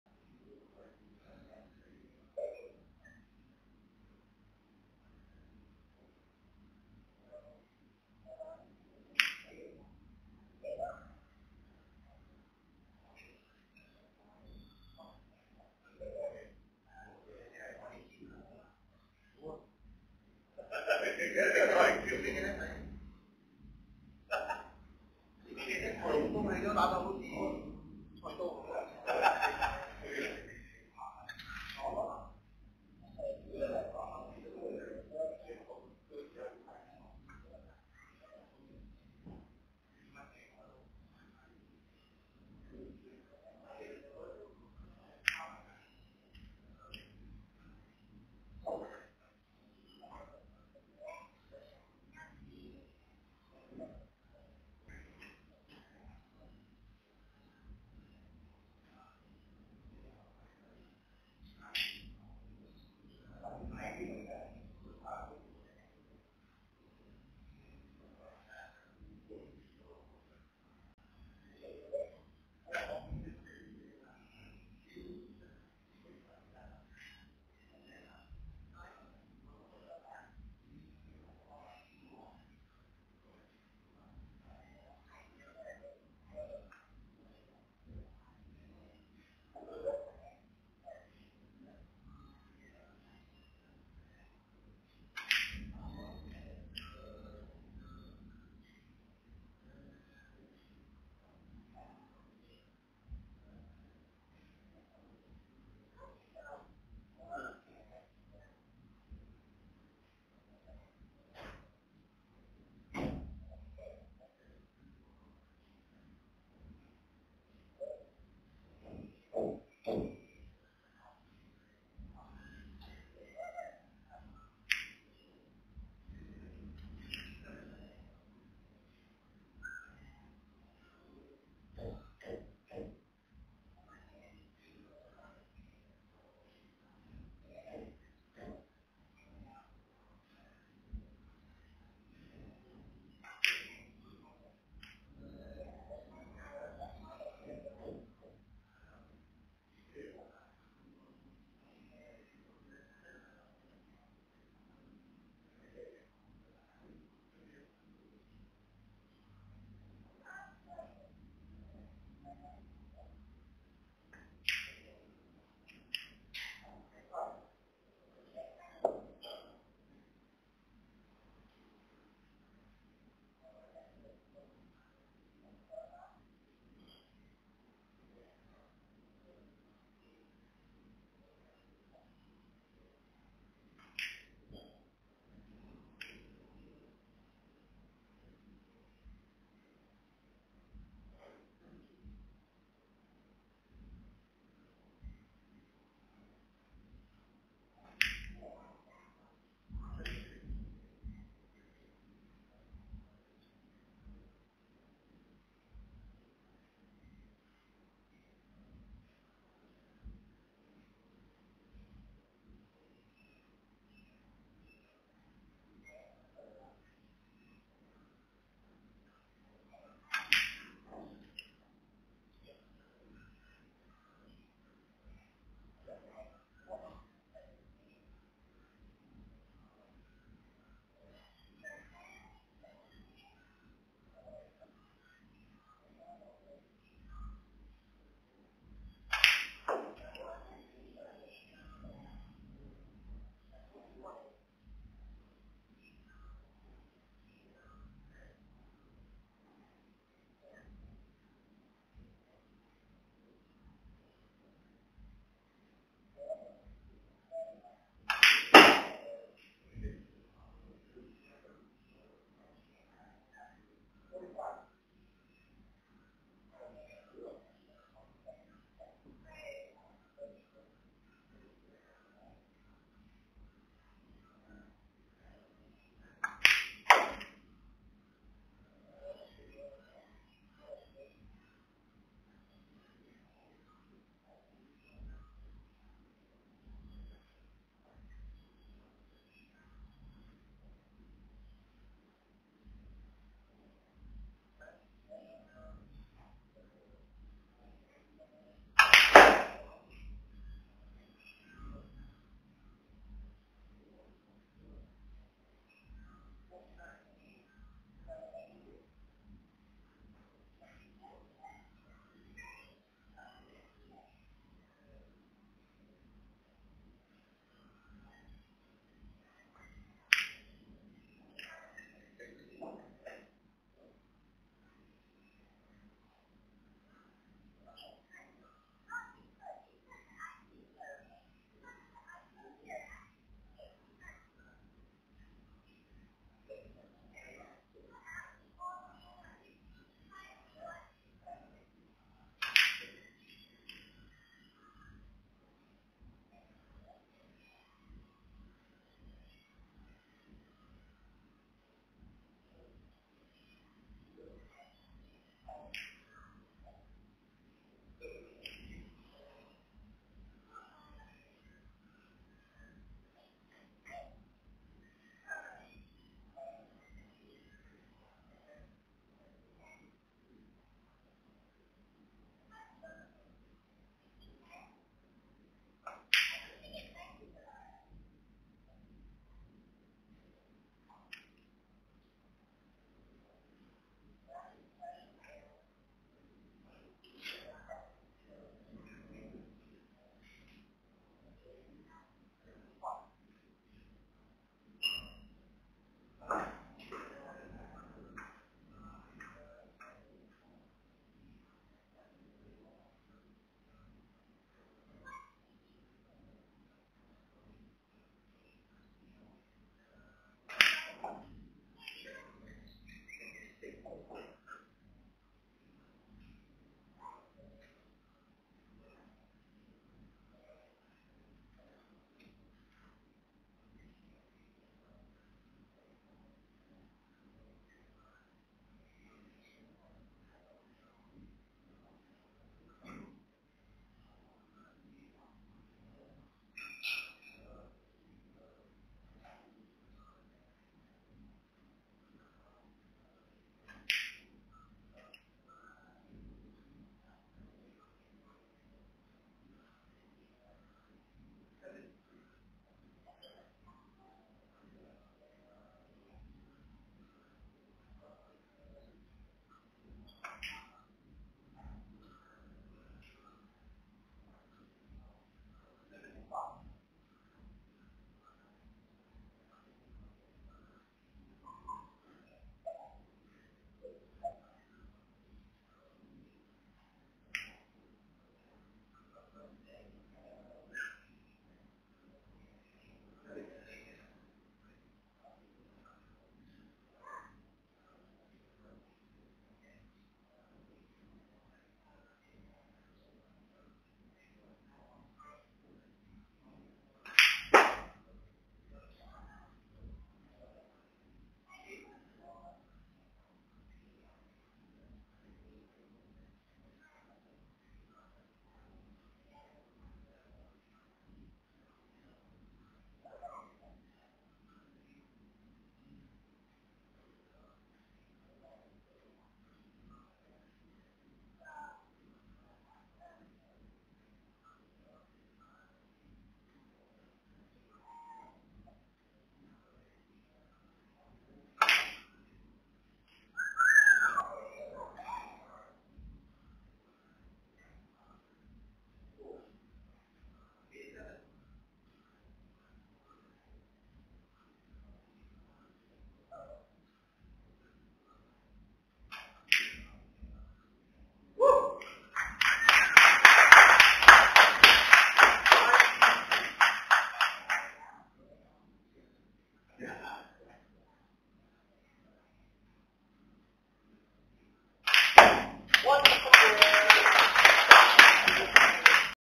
Malaysia Snooker. Rory Thor Chuan Leong 100 Break vs JH Leong (18/12/2021)
Rory Thor Chuan Leong having a sparring session with former coach JH Leong.